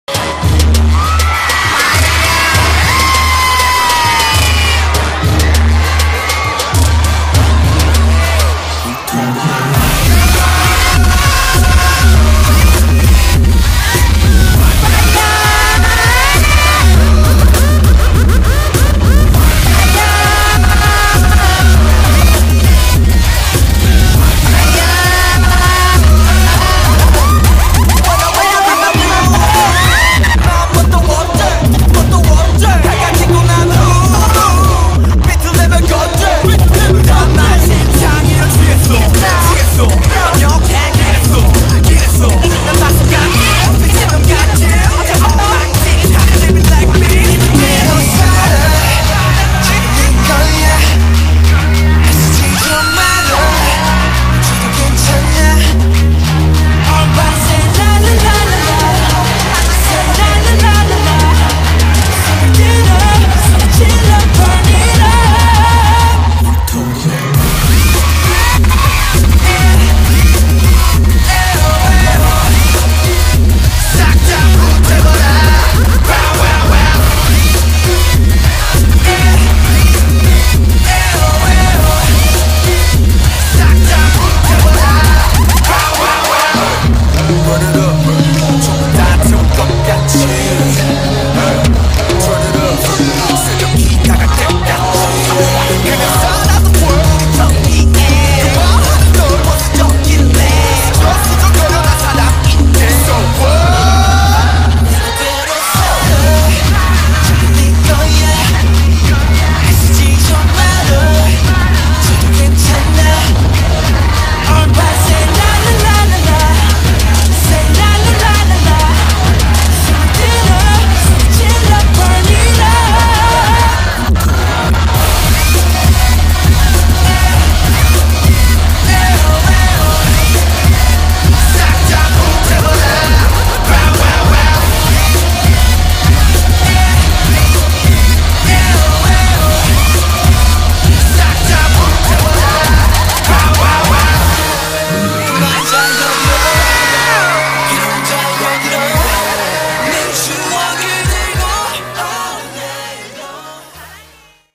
07102016 WTF dance cover bts di K-CONTEN EXPO
Kakak-kakak ini dari bandung loh...  Mereka keren abis ya...